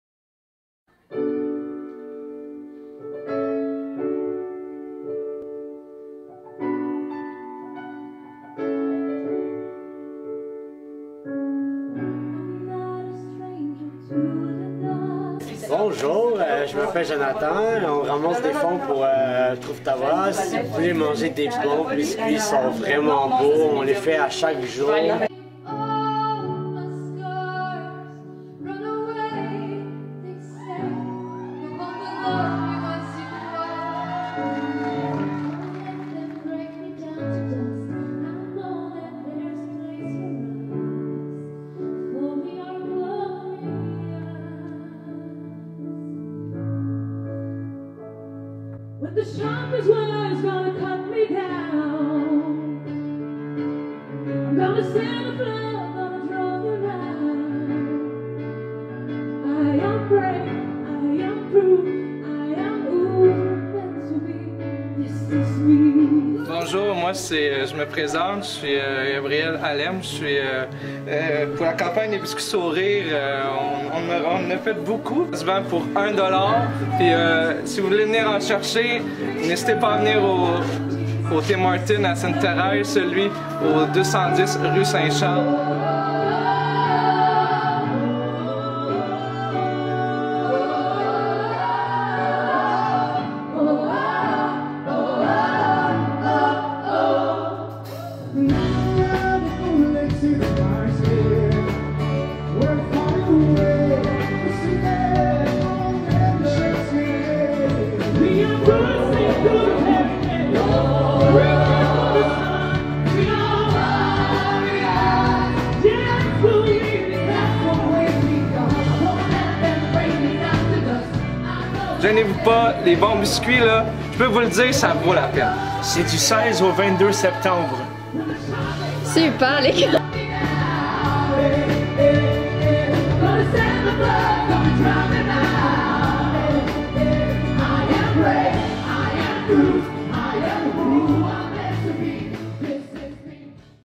Campagne de biscuit sourire
voici un video de notre campagne de biscuit sourire  au profit de l'academie des arts trouve ta voie